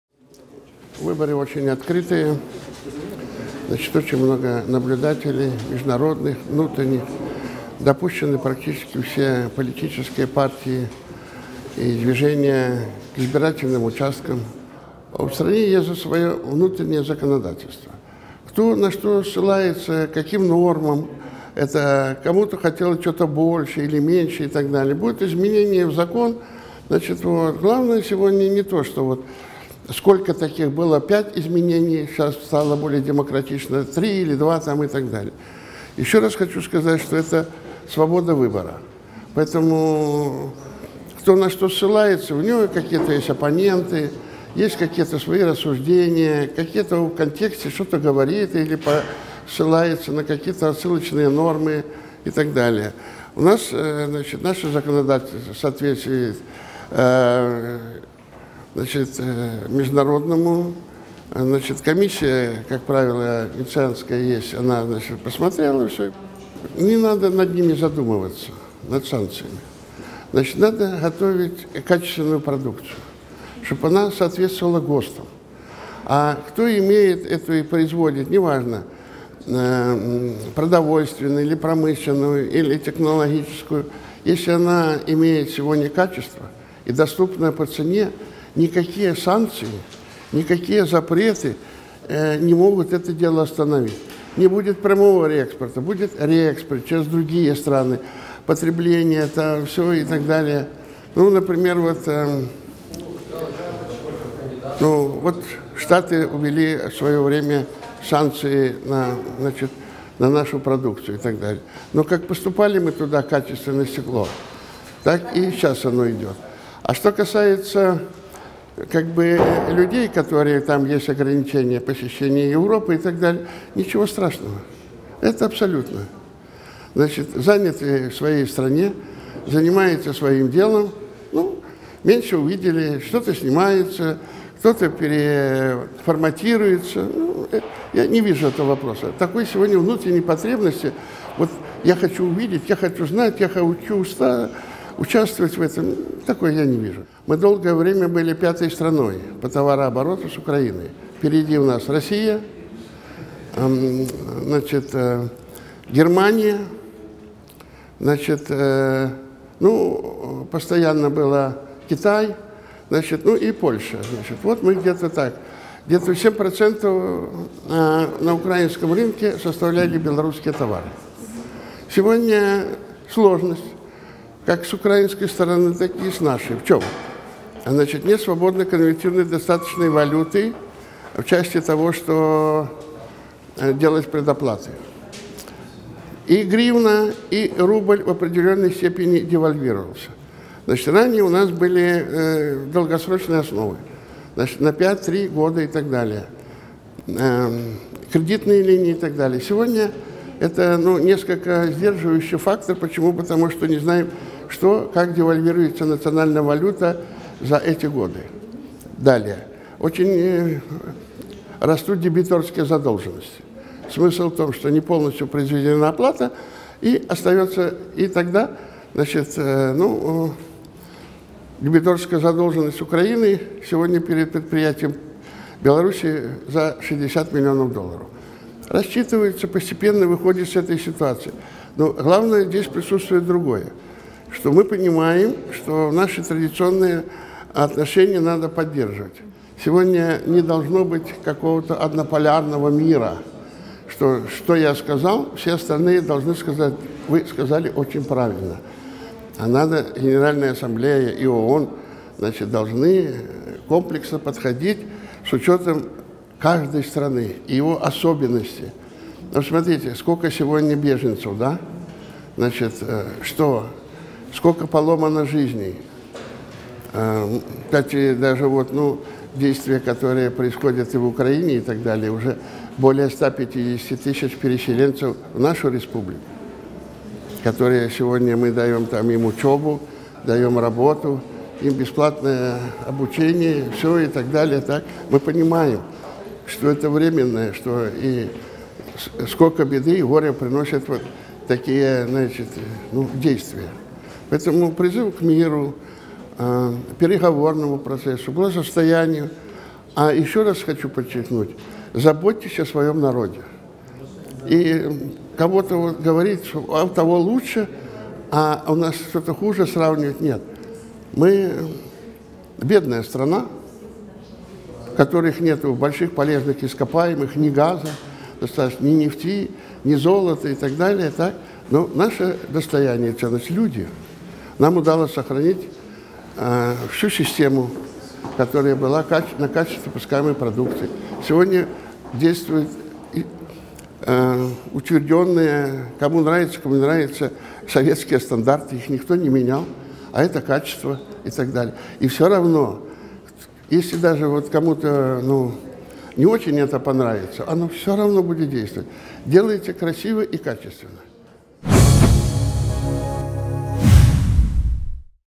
Интервью с послом Республики Беларусь
В преддверии президентских выборов Валентин Величко – посол Республики Беларусь дал интервью 17 каналу, рассказав о своем видении выборов в Республики Беларусь, торгово-экономических отношениях между странами Украина-Беларусь.